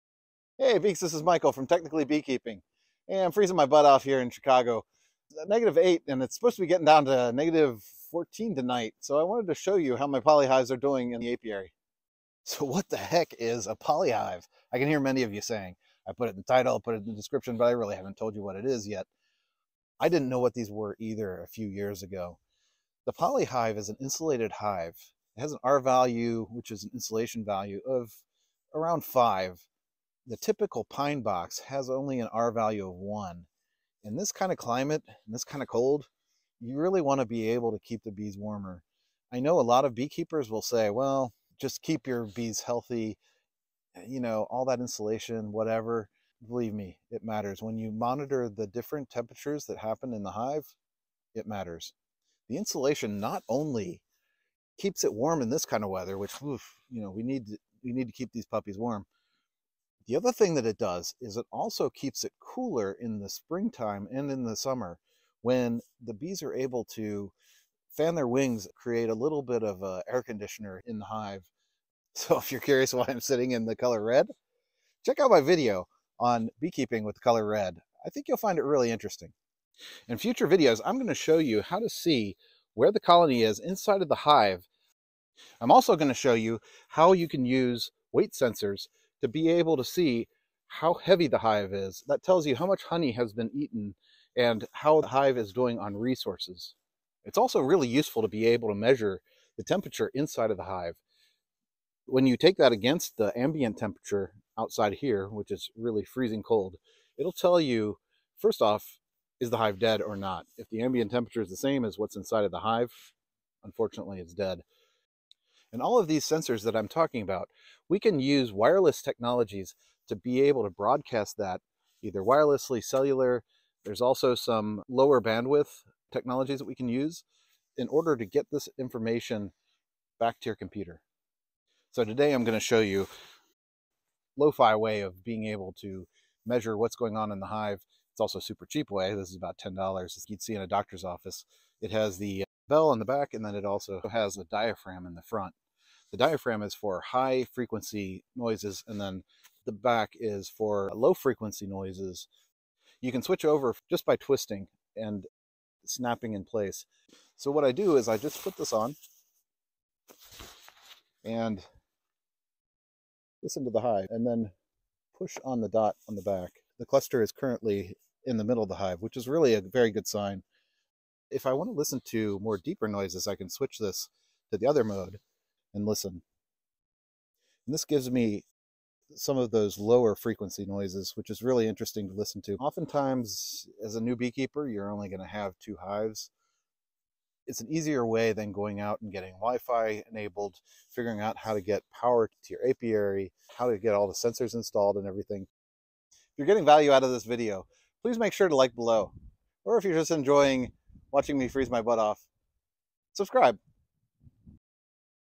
Frozen Fortress: honeybees conquer -8°F blizzard in poly hive sanctuary!
Buried alive in a foot of snow and -8°F temperatures, the honey bees cluster for survival. Insulated hives to the rescue.  See how my wellness check fairs in this episode.